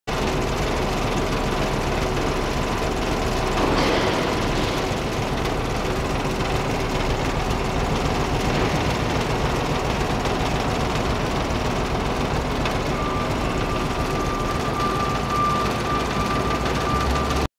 Bulk Material Handling FulFiller trajectory trial
Redler / Schenck FulFiller container loading system trajectory trial throwing animal feed.